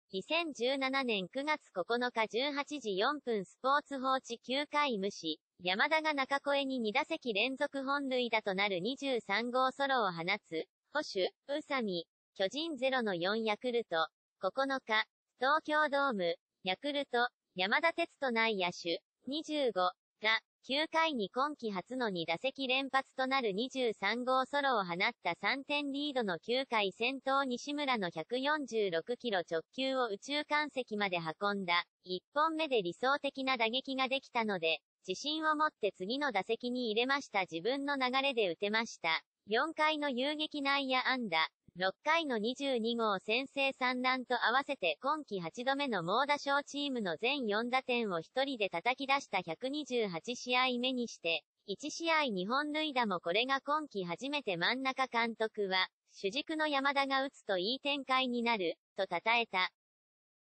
【ヤクルト】山田哲人が今季初の２打席連続弾！「自分の流れで打てました」- ニュース 2017年9月10日
2017年9月9日18時4分  スポーツ報知 ９回無死、山田が中越えに２打席連続本塁打となる２３号ソロを放つ（捕手・宇佐見） 　◆巨人０―４ヤクルト（９日・東京ドーム） 　ヤクルト・山田哲人内野手（２５）が、９回に今季初の２打席連発となる２３号ソロを放った。 　３点リードの９回先頭。西村の１４６キロ直球を右中間席まで運んだ。「１本目で理想的な打撃ができたので、自信を持って次の打席に入れました。自分...